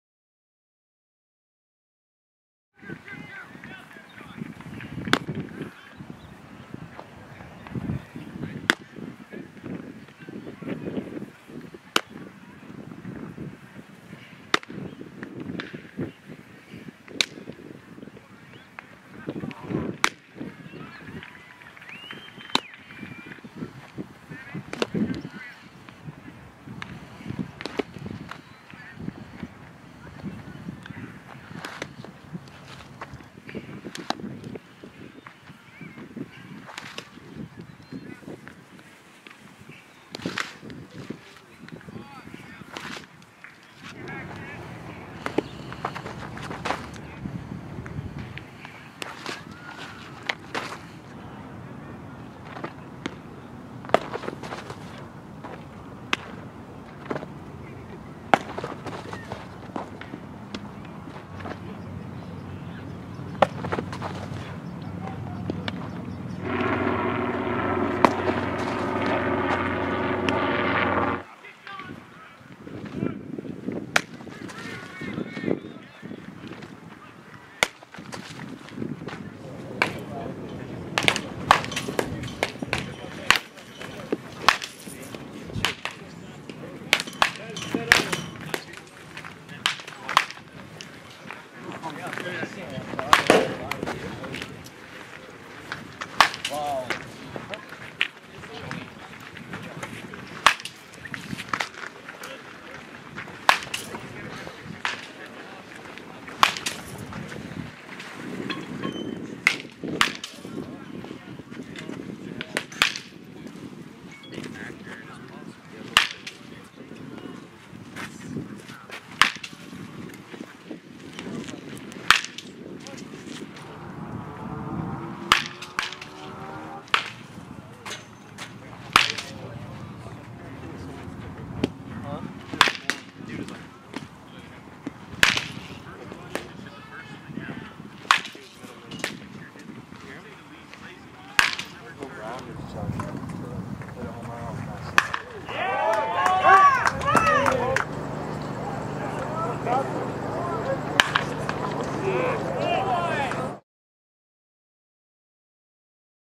Grant Decatrel Prospect Video, C, West Torrance High School Class of 2018
Decatrel receiving, blocking and pops at AZ Junior Fall Classic